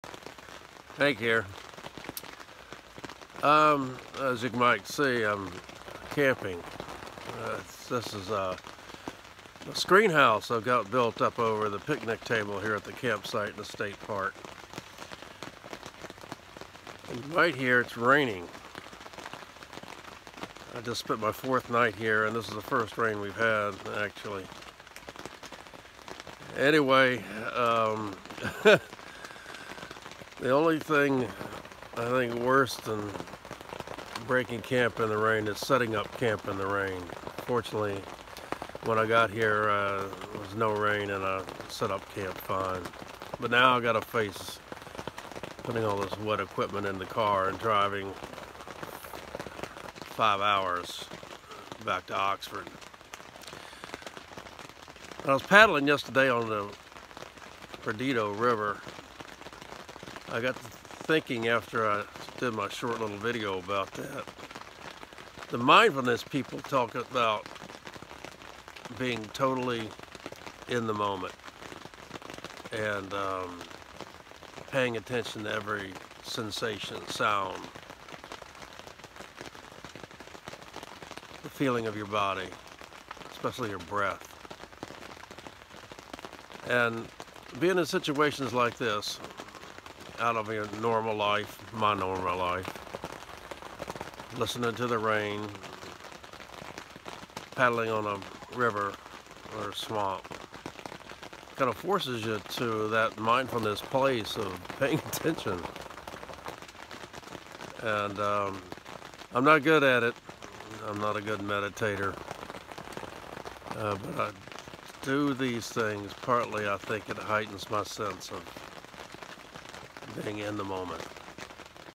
Camping in the rain—mindfulness
Sitting in a tent, listening to the rain, forces me to be mindful of the moment.